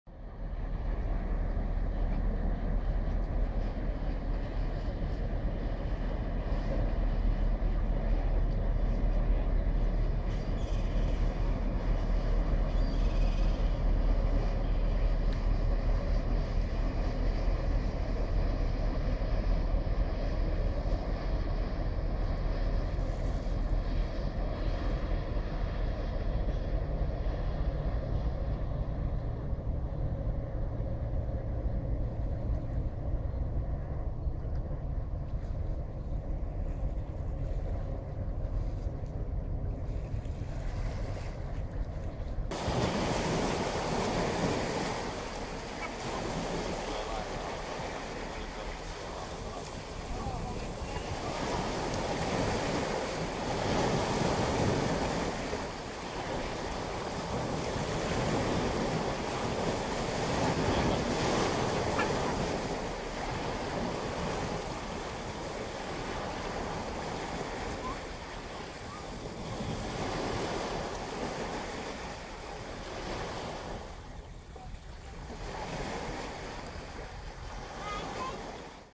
Barnacle Geese Threatened by the Wake of a Huge Ferry Boat
I witnessed this incident during my short visit to the capital of Finland, Helsinki. As I didn't have my DSLRs with me, I documented the incident on my Lumia 1020, which, however, has  a great video camera for a mobile.